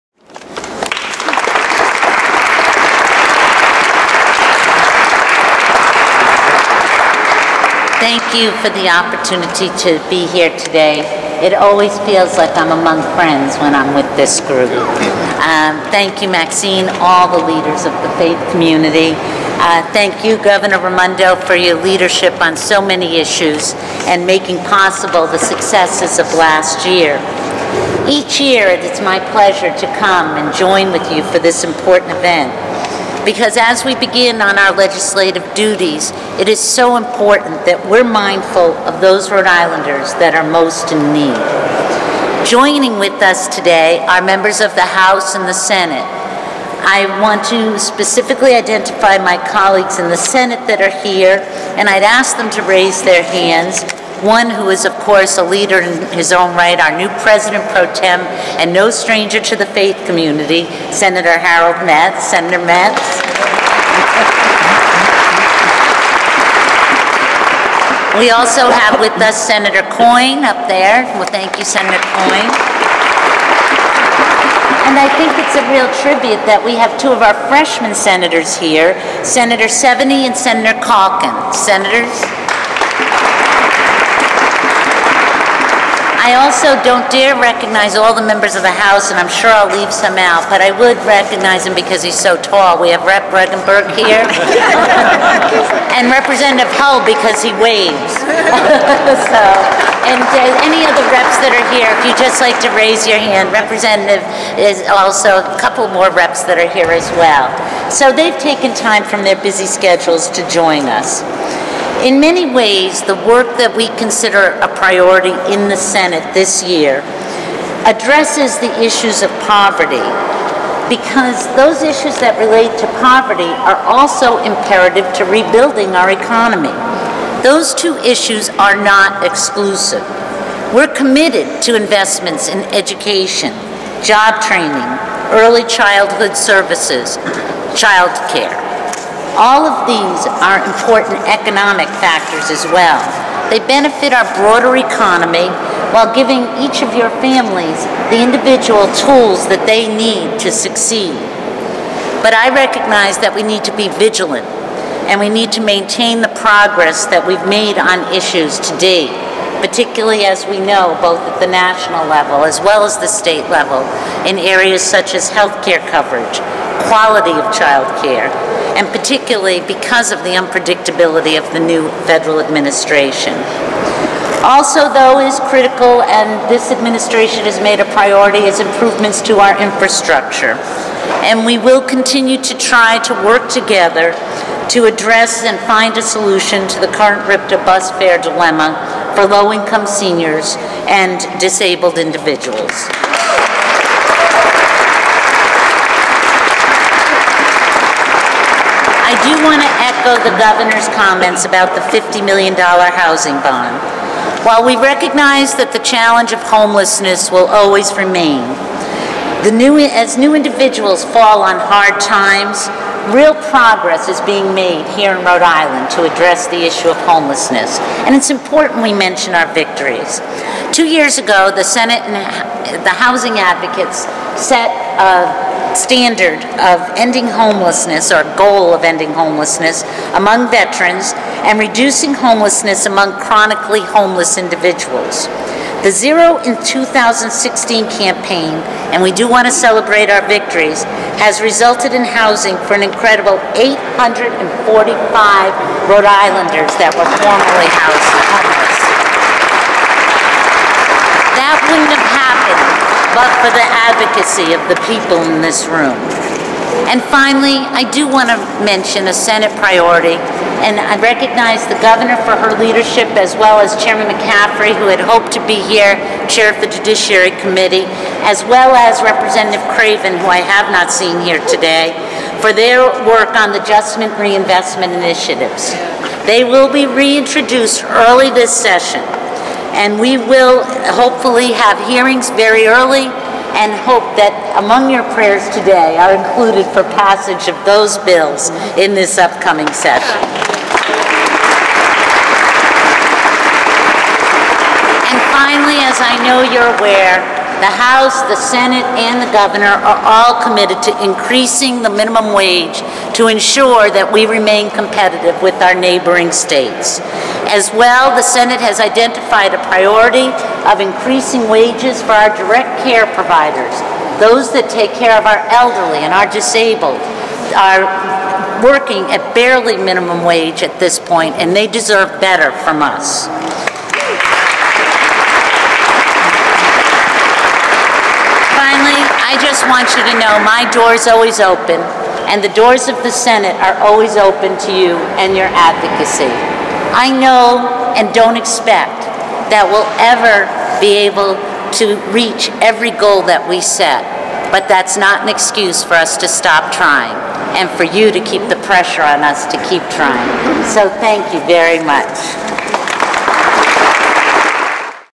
Faith Leaders Unite to Combat Poverty at RI State House
The 2017 Fighting Poverty with Faith Vigil at the Rhode Island State House brought together community leaders, legislators, and faith-based advocates to address poverty-related issues in the state. The event featured remarks from prominent figures, including Governor Gina Raimondo, who highlighted her administration’s efforts to combat homelessness and improve housing accessibility. Senate leaders, such as Senate President Pro Tempore Harold Metts and Senators Josh Miller and Jeanine Calkin, were also in attendance, alongside House Representatives, including Representatives Arthur Handy and David Bennett.

The discussion centered on legislative priorities aimed at reducing poverty, including investments in education, job training, early childhood services, and childcare. Governor Raimondo emphasized the importance of the $50 million housing bond and progress made through initiatives like the Zero: 2016 campaign, which successfully housed 845 Rhode Islanders. The event also addressed challenges such as RIPTA bus fare affordability for low-income seniors and disabled individuals, as well as efforts to increase the minimum wage and improve wages for direct care providers.

The vigil underscored the collaborative efforts between state leadership and community advocates to address systemic issues and maintain progress in the face of federal uncertainty. Lawmakers expressed their commitment to continuing advocacy and legislative action to support Rhode Island’s most vulnerable populations.